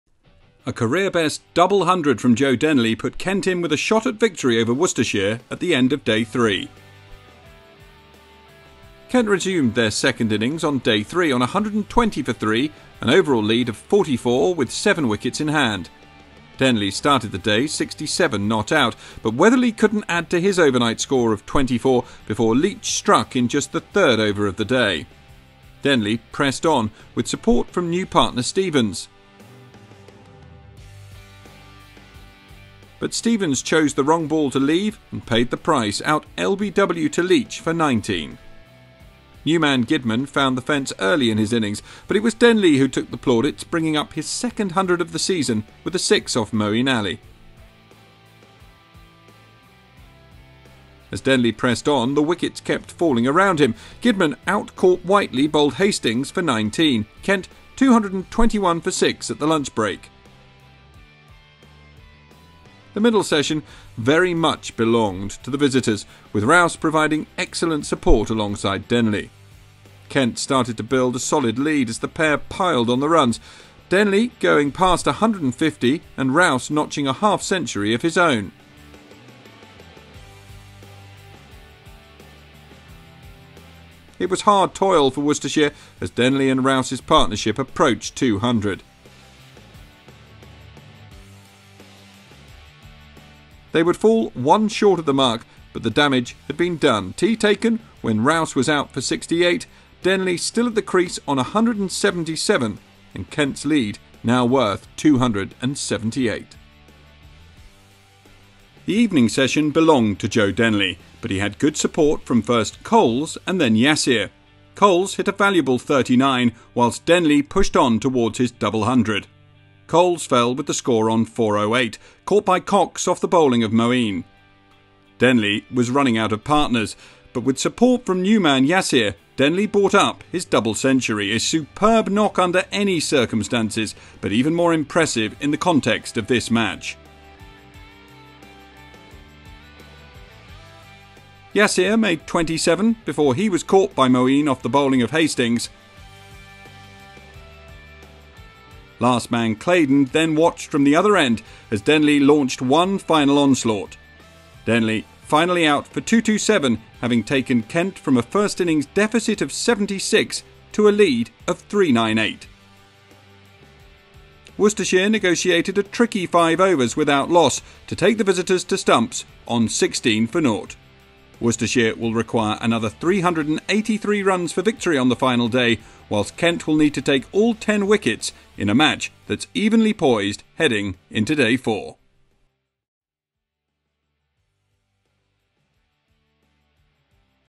Joe Denly hits career-best 227 at Worcester
Highlights of day three of Worcestershire v Kent, Specsavers County Championship